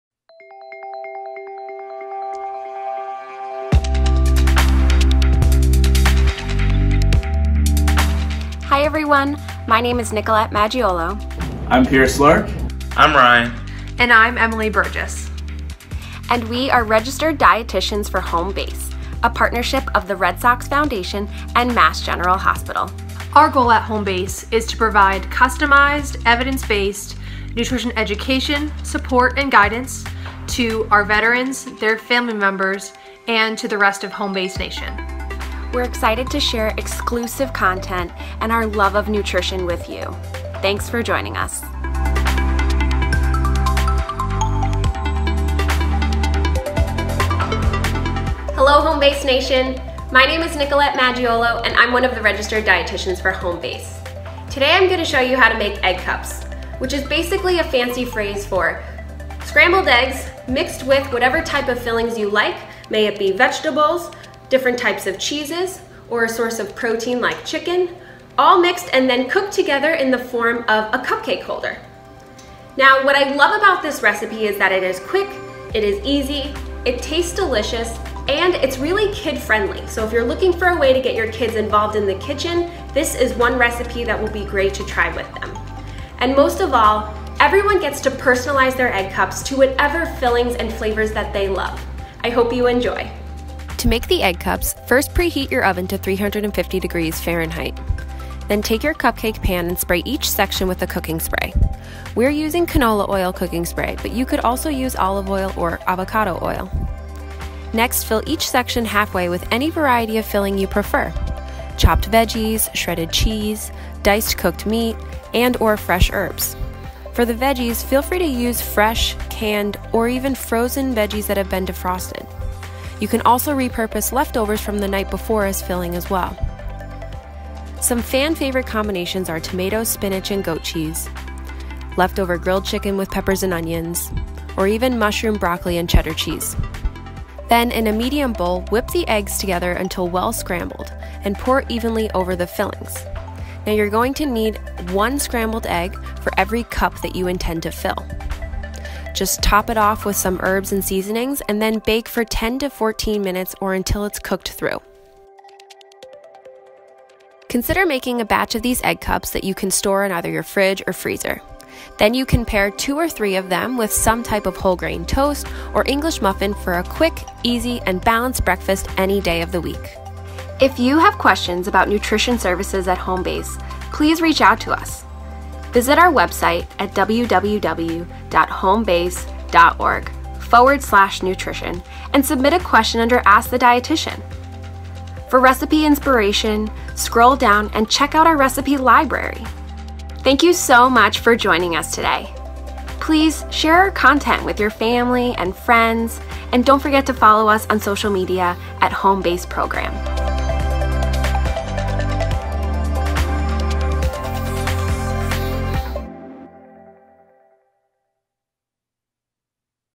Nutrition: Egg Cup Recipe for the Entire Family
What’s cooking in your kitchen these days? Come inside our Home Base kitchen and join Nicolette Maggiolo as for an egg-cellent HBHealthAtHome cooking demo!

These egg cups are a great kid-friendly recipe to try at home. Loaded with protein and fiber, these egg cups that will keep everyone full throughout the morning. Prepared in bulk, these egg cups can be an on-the-go breakfast item throughout the week.

The best part - everyone in the family can personalize their fillings to whatever flavors they love most!